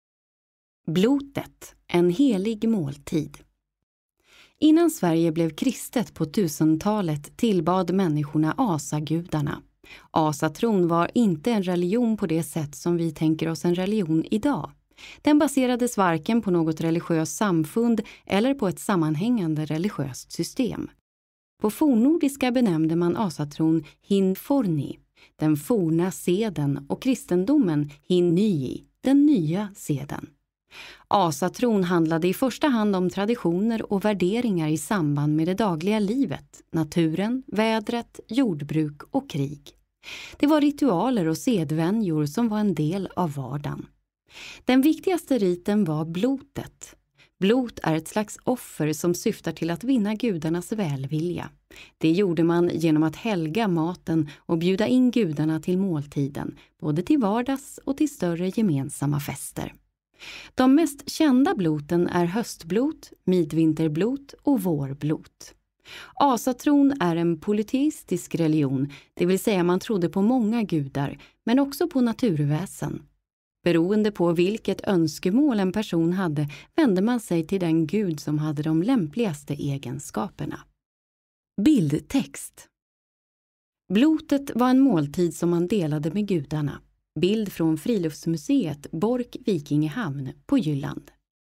9 Blotet - en helig måltid skylttext
Den här ljudfilmen är en av flera audioguider som hör till Badelundaåsens natur- och kulturstig vid Anundshög öster om Västerås. Läs mer på